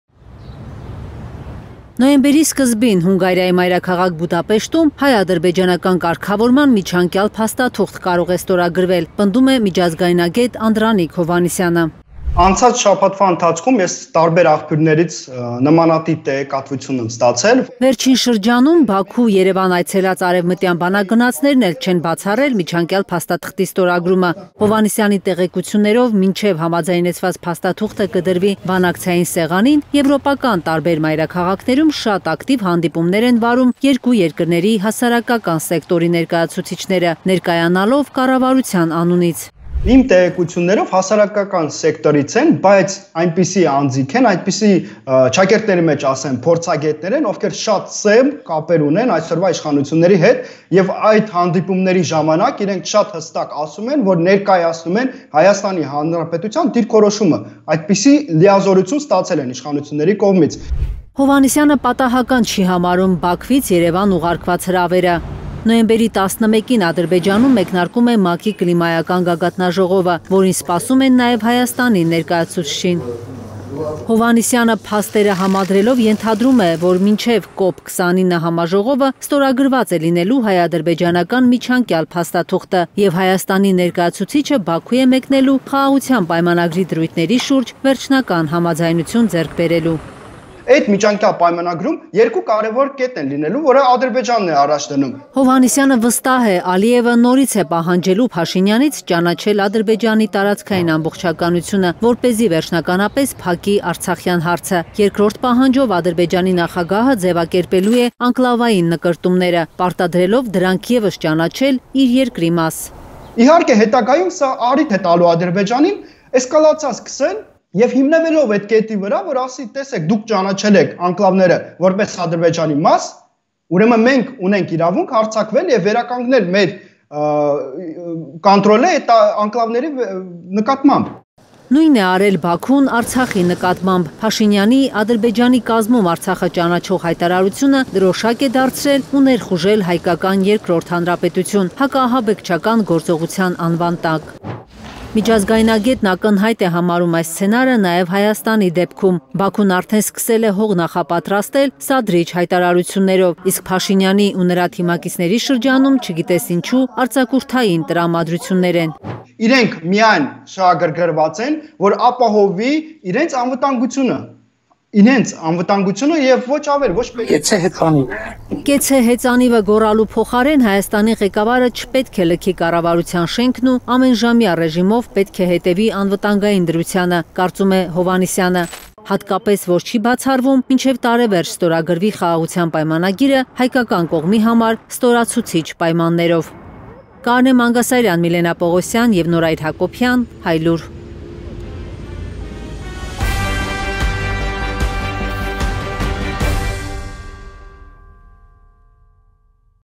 Հայ-ադրբեջանական կարգավորման միջանկյալ փաստաթուղթ կստորագրվի՞ նոյեմբերի սկզբին
Խաղաղության պայմանագրից առաջ Հայաստանն ու Ադրբեջանը, հնարավոր է, միջանկյալ փաստաթուղթ ստորագրեն: Այսպիսի տեղեկություններ ունի միջազգայնագետ Անդրանիկ Հովհաննիսյանը: Ըստ նրա՝ այս պահին Հայաստանի և Ադրբեջանի քաղհասարակության տարբեր ներկայացուցիչներ քննարկում են միջանկյալ փաստաթղթի բովանդակությունը, միջազգայնագետն էլ հաջողել է, որոշ մանրամասներ ստանալ: Այսպես՝ ստորագրվելիք միջանկյալ փաստաթղթում ադրբեջանցիները բարձրացնում են տարածքային ամբողջականության ճանաչման՝ անկլավների հարցերը: Հովհաննիսյանը պնդում է՝ հայ և ադրբեջանցի գործիչները պայմանավորվածություն ունեն աշխատանքները ավարտել Բաքվում նոյեմբերի 11-ին սկսվող ՄԱԿ-ի կլիմայական գագաթնաժողովից առաջ: